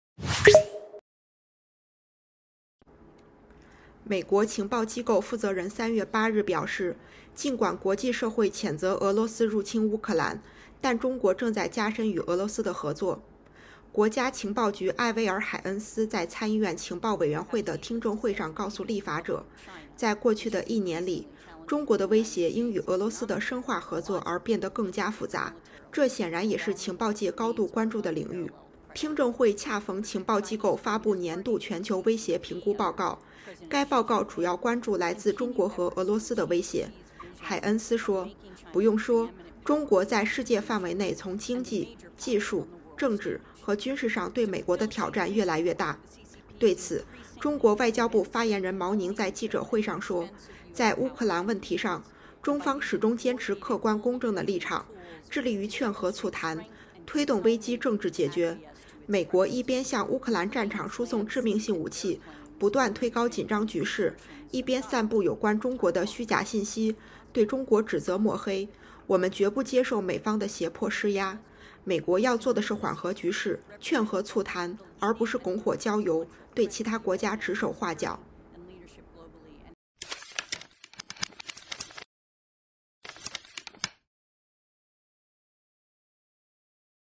美国情报机构声称中国正在深化与俄罗斯的合作
美国情报机构负责人3月8日表示，尽管国际社会谴责俄罗斯入侵乌克兰，但中国正在加深与俄罗斯的合作。

国家情报局艾薇儿海恩斯在参议院情报委员会的听证会上告​​诉立法者：“在过去的一年里，中国的威胁因与俄罗斯的深化合作而变得更加复杂，这显然也是情报界高度关注的领域。”

听证会恰逢情报机构发布年度全球威胁评估报告，该报告主要关注来自中国和俄罗斯的威胁。

海恩斯说：“不用说，中国在世界范围内从经济、技术、政治和军事上对美国的挑战越来越大。”

对此，中国外交部发言人毛宁在记者会上说：“在乌克兰问题上，中方始终坚持客观公正的立场，致力于劝和促谈，推动危机政治解决。美国一边向乌克兰战场输送致命性武器，不断推高紧张局势，一边散布有关中国的虚假信息，对中国指责抹黑。我们绝不接受美方的胁迫施压。美国要做的是缓和局势、劝和促谈，而不是拱火浇油，对其他国家指手画脚。”